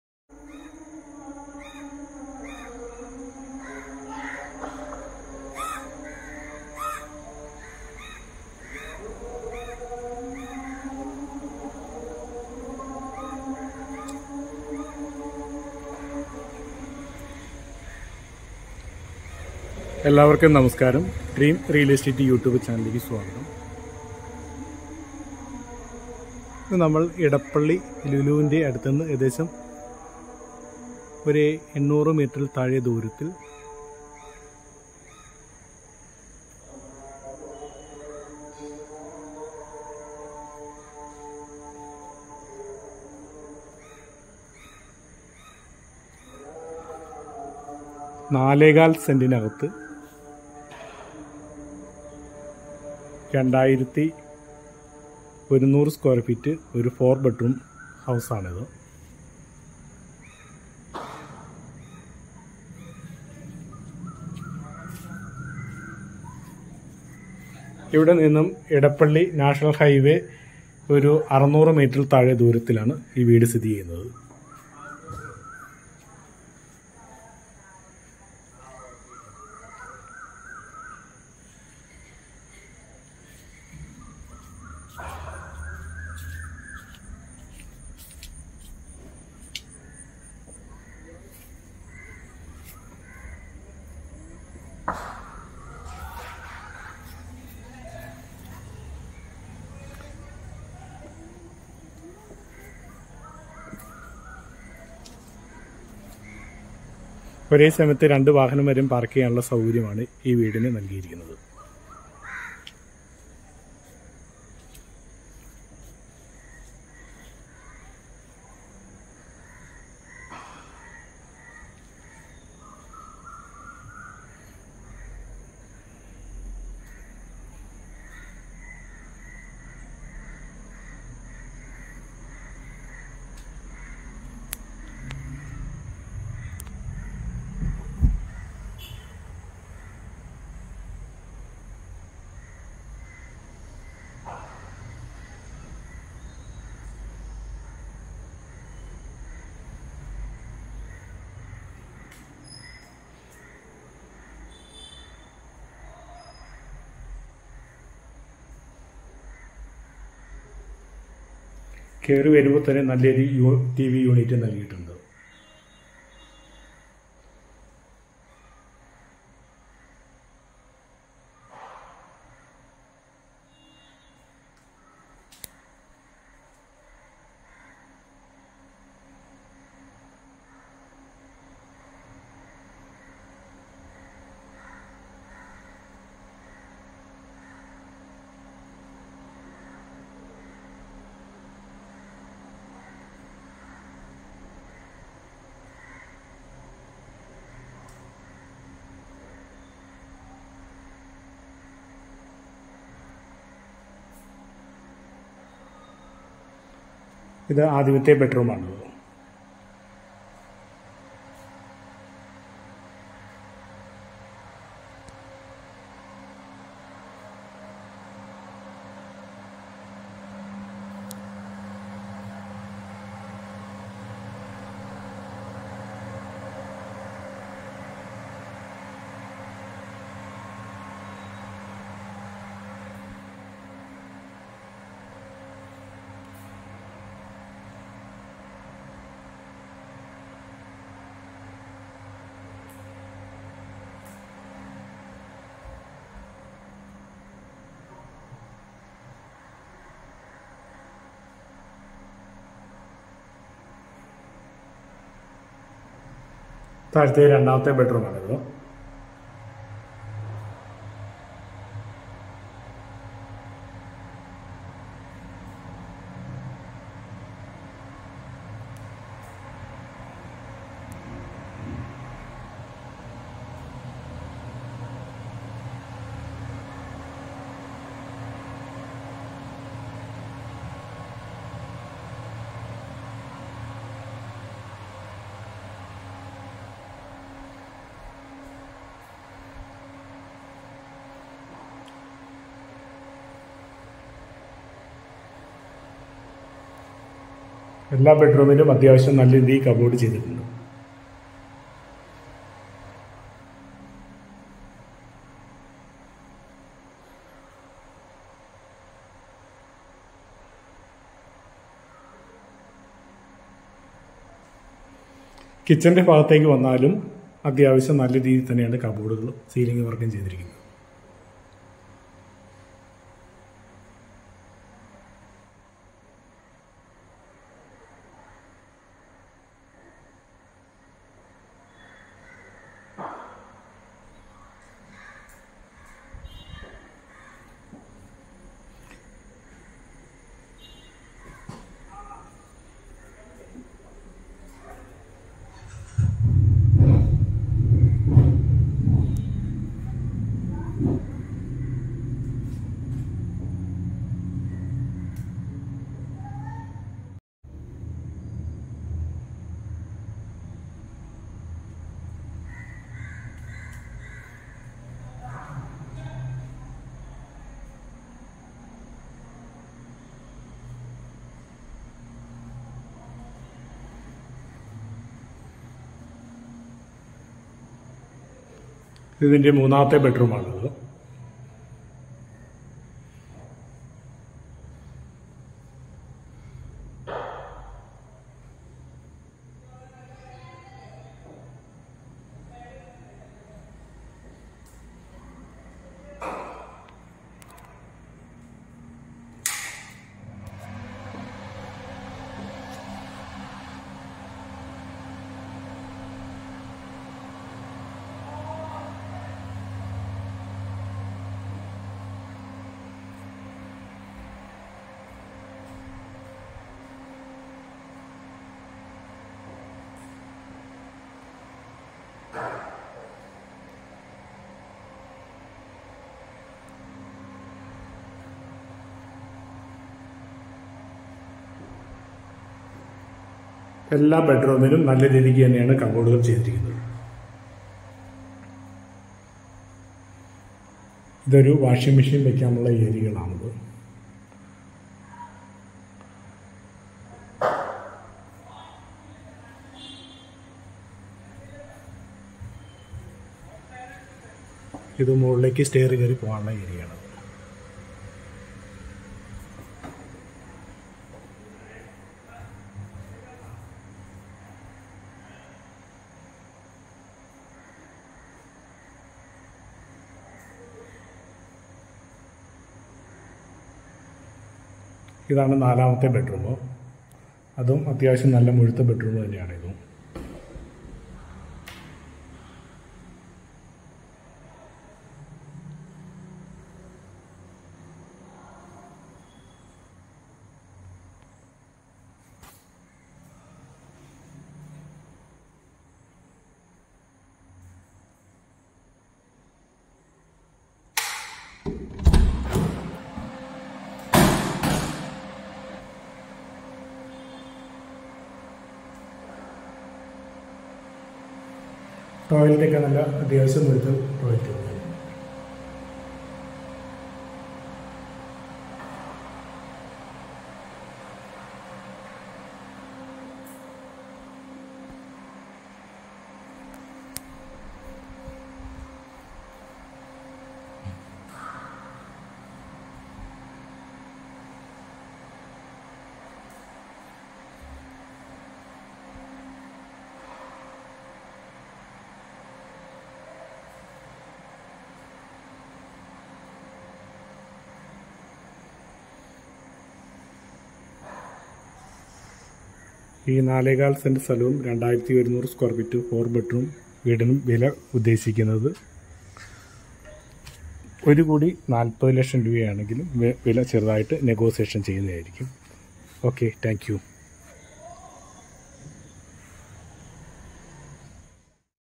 INDEPENDENT NEW HOUSE FOR SALE SALE AT ERNAKULAM, EDAPALLY
4.25 CENT 2100SQFT 4 BEDROOM INDEPENDENT NEW HOUSE FOR SALE SALE AT
ERNAKULAM, EDAPALLY

850M FROM LULU MALL

700M FROM NH47

ASKING PRICE 1CR 40 LAKHS